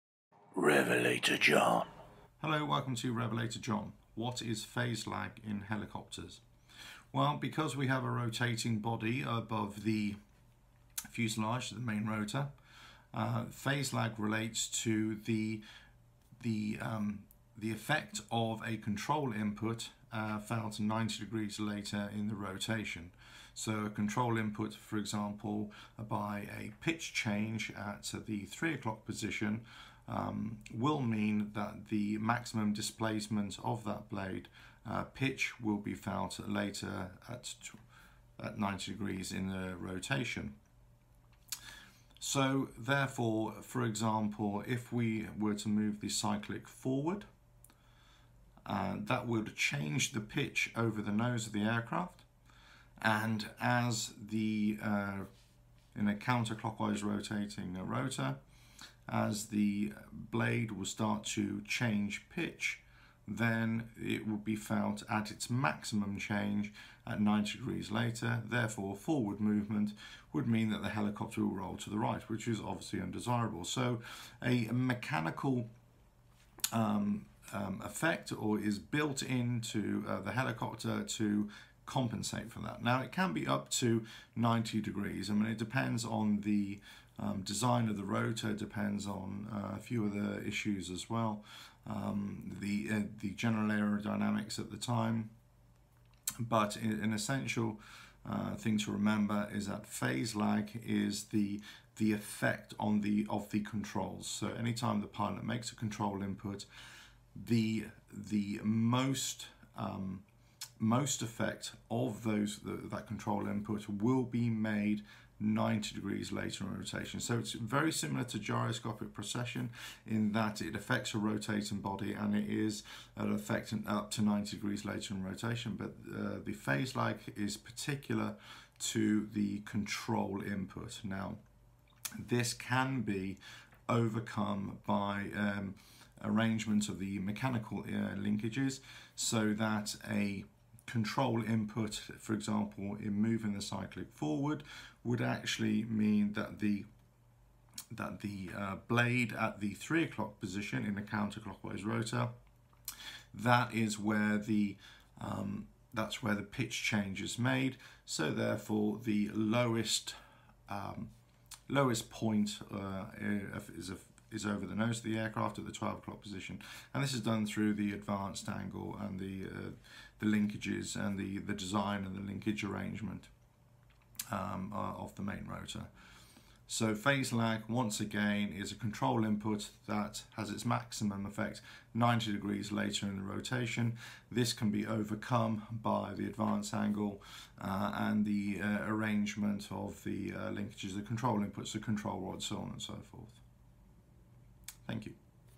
What is Phase Lag in Helicopters?
A simplified view of aviation theory - What is Phase Lag in Helicopters?